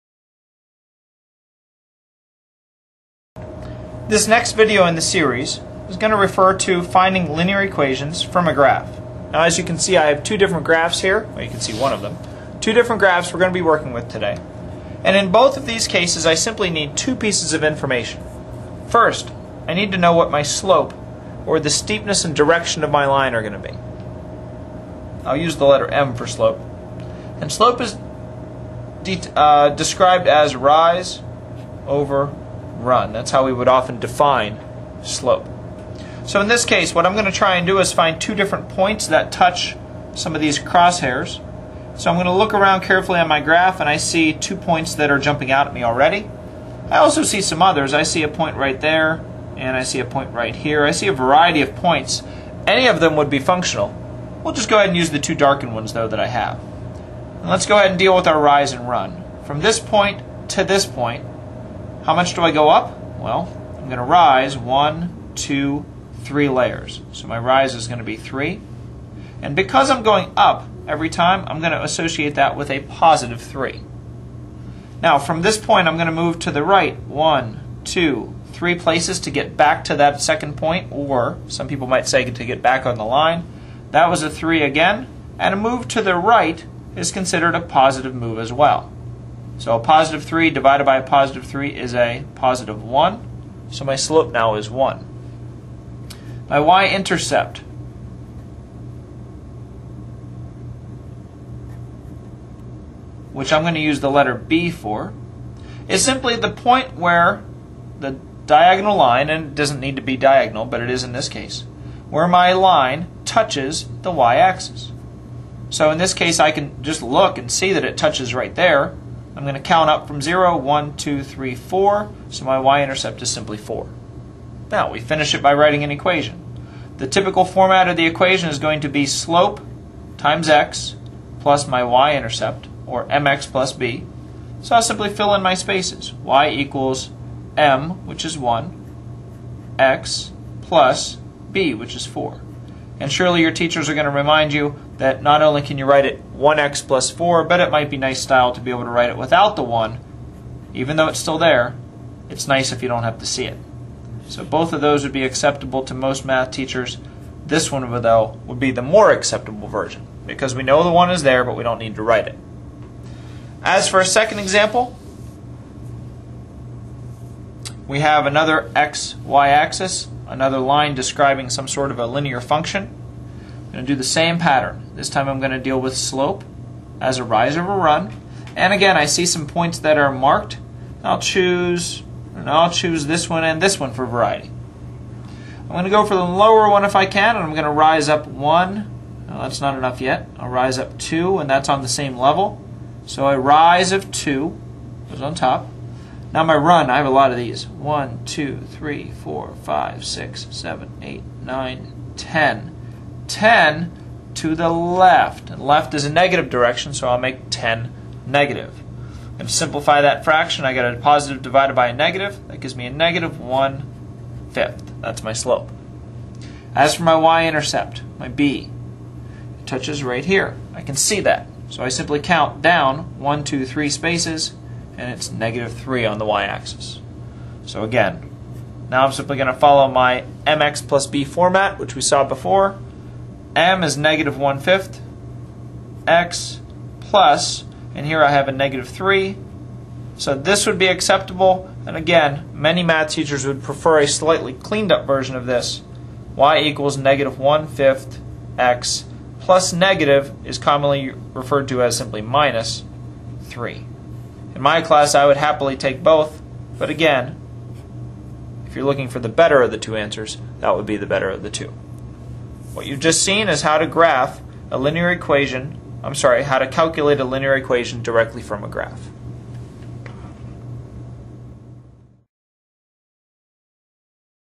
Determine an equation from a graph
Given a straight line graph (linear graph), this video will show you how to read the slope and the y-intercept directly from the graph.  Useing slope and y-intercept, an equation can then be written to represent the graph.